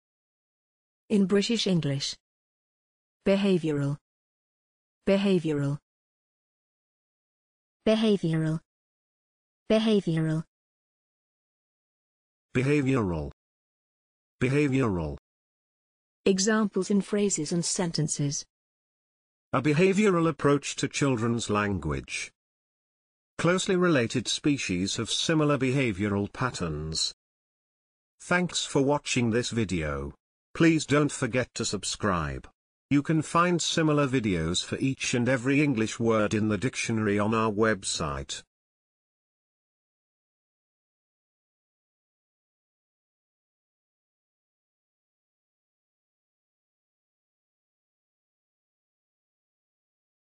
behavioural - pronunciation + Examples in sentences and phrases
This is the pronunciation of behavioural in British English + a few examples of its pronunciation in phrases and sentences.